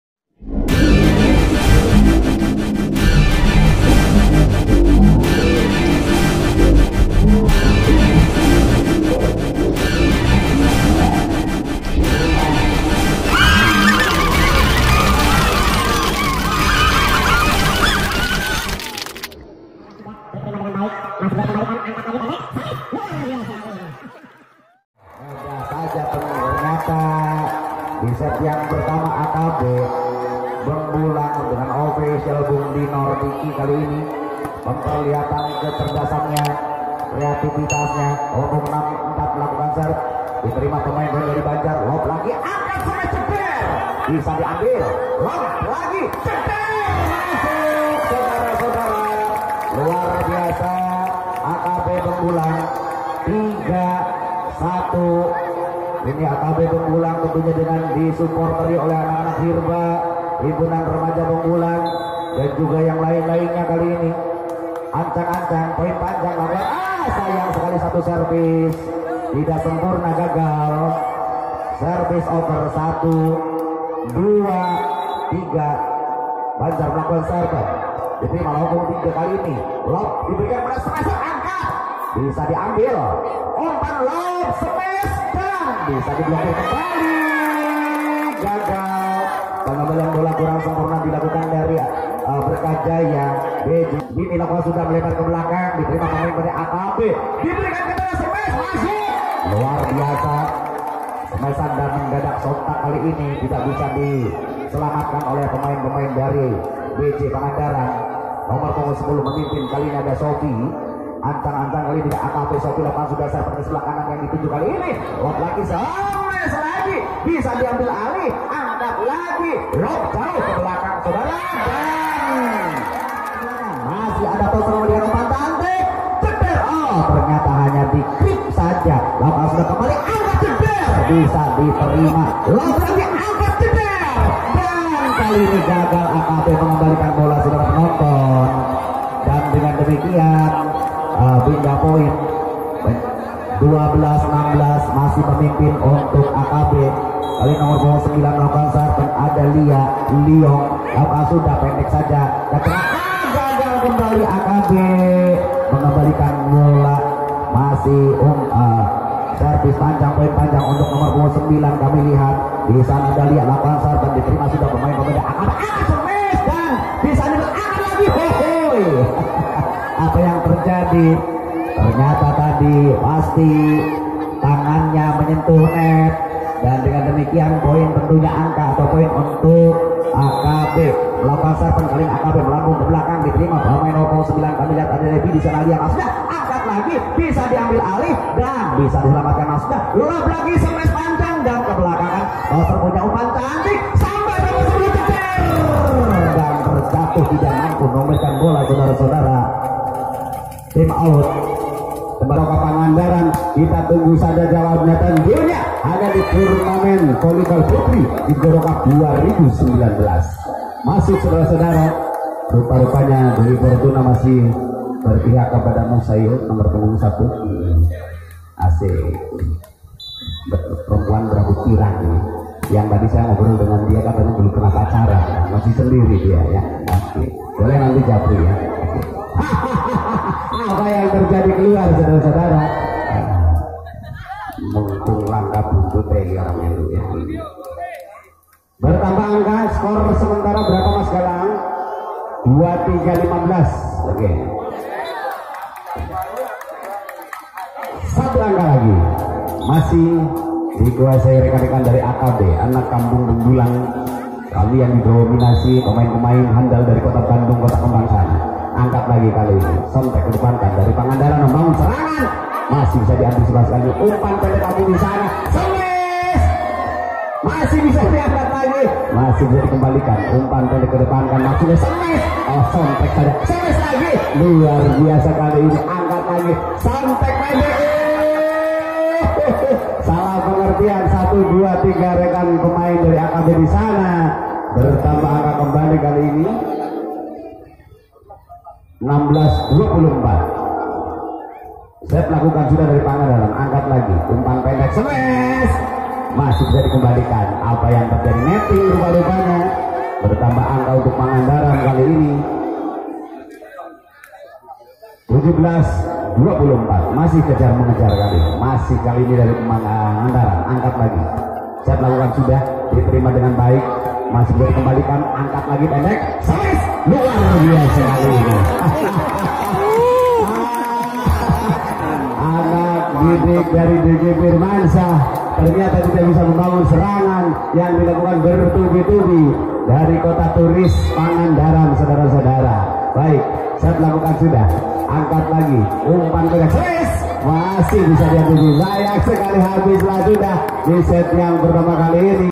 DAHSYAT Detik-detik smash menyasar muka di turnamen bola voli putri pemain PON JABAR
DAHSYAT Detik-detik smash menyasar muka di turnamen bola voli putri Grgorao Cup
Turnamen volly putri Gegoro Cup 2019